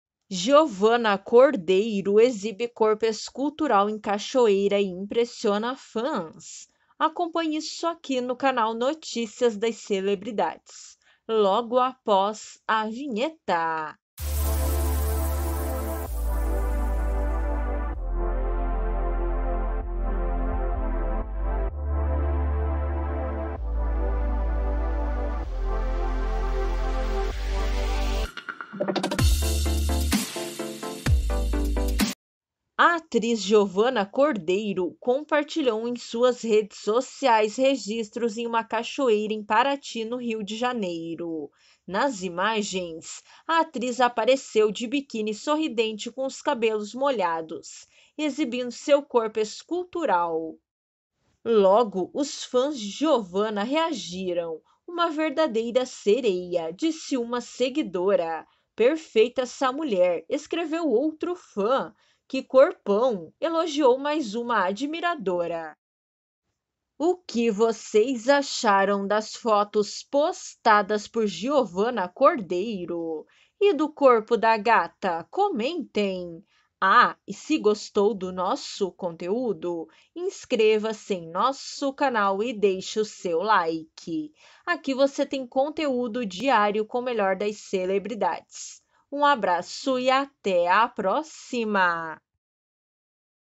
GIOVANA CORDEIRO EXIBE CORPO ESCULTURAL EM CACHOEIRA E IMPRESSIONA FÃS!
Nas imagens, a atriz apareceu de biquíni, sorridente, com os cabelos molhados, exibindo seu corpo escultural.

Logo, os fãs de Giovana reagiram: "Uma verdadeira sereia", disse uma seguidora; "Perfeita essa mulher", escreveu outro fã; "Que corpão", elogiou mais uma admiradora.

Assistam o vídeo até o final e comentem o que vocês acharam das fotos postadas por Giovana Cordeiro! E do corpo da gata? Comentem!